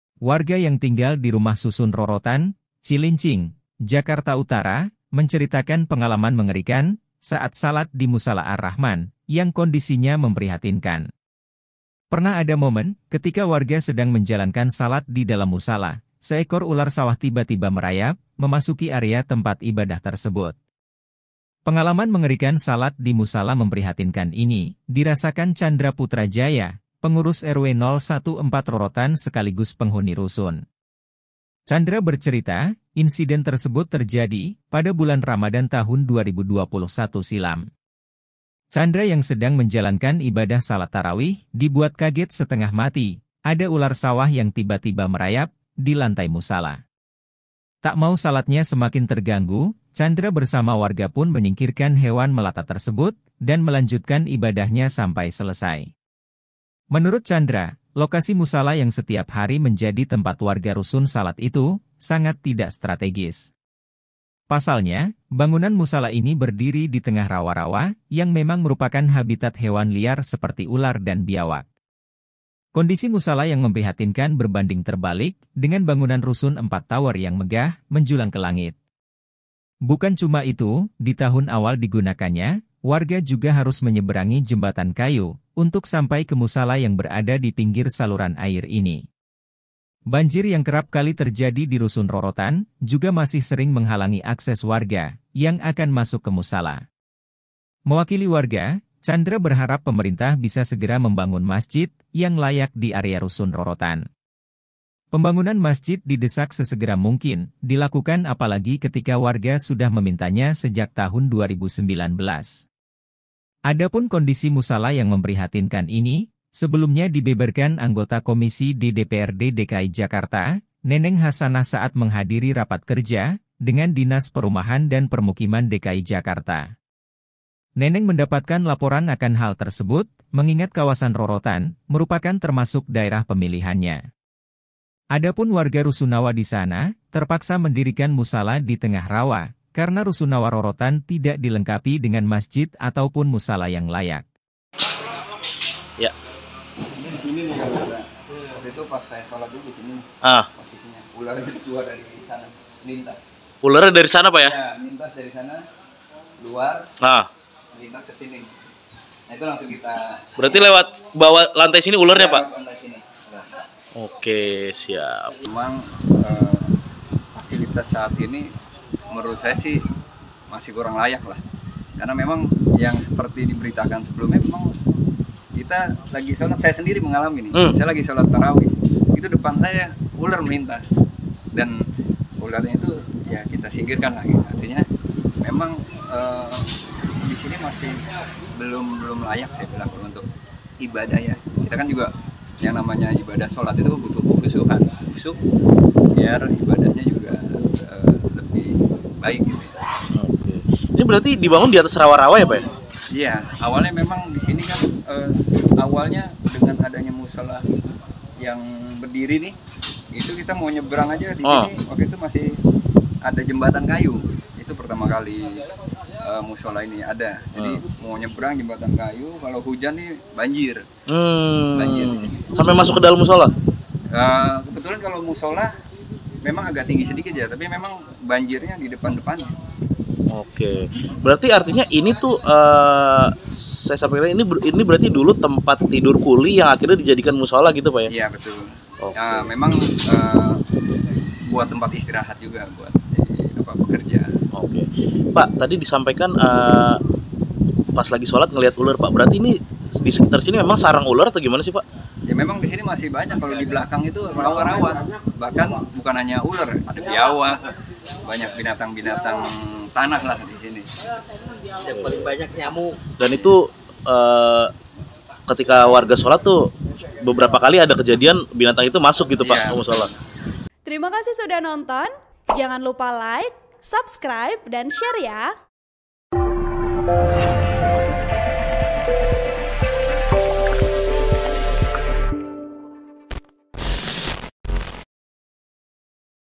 Bikin Merinding, Mau Salat Jemaah Malah Lihat Ular Masuk Musala Rusun Rorotan hingga Kaget
TRIBUN-VIDEO.COM - CILINCING - Warga yang tinggal di Rumah Susun (Rusun) Rorotan, Cilincing, Jakarta Utara menceritakan pengalaman mengerikan saat salat di Musala Ar-Rahman yang kondisinya memprihatinkan.

Pernah ada momen ketika warga sedang menjalankan salat di dalam musala, seekor ular sawah tiba-tiba merayap memasuki area tempat ibadah tersebut.

Pengalaman mengerikan salat di musala memprihatinkan ini dirasakan Chandra Putra Jaya, pengurus RW 014 Rorotan sekaligus penghuni rusun.

Chandra bercerita, insiden tersebut terjadi pada bulan Ramadan tahun 2021 silam.

Chandra yang sedang menjalankan ibadah salat tarawih dibuat kaget setengah mati ada ular sawah yang tiba-tiba merayap di lantai musala.

Tak mau salatnya semakin terganggu, Chandra bersama wargapun menyingkirkan hewan melata tersebut dan melanjutkan ibadahnya sampai selesai.

“Saya lagi salat tarawih itu di depan saya ada ular melintas dan ularnya itu kita singkirkan lah, artinya memang di sini masih belum layak lah,” kata Chandra saat ditemui TribunJakarta.com di lokasi, Rabu (12/7/2023).

Menurut Chandra, lokasi musala yang setiap hari menjadi tempat warga rusun salat itu sangat tidak strategis.

Pasalnya, bangunan musala ini berdiri di tengah rawa-rawa yang memang merupakan habitat hewan liar seperti ular dan biawak.

Kondisi musala yang memprihatinkan berbanding terbalik dengan bangunan rusun empat tower yang megah, menjulang ke langit.